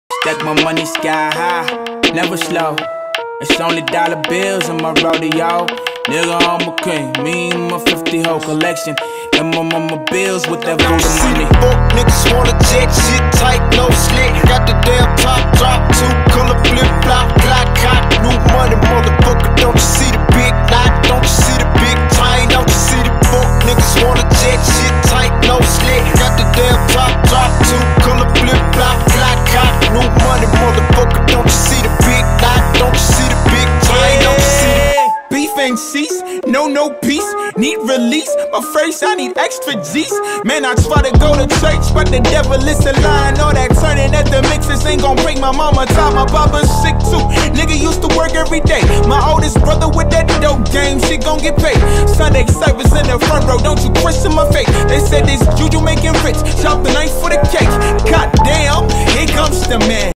Twerk and Travel in Italy (Sardinia)
I used that beautiful location to create a little concept video.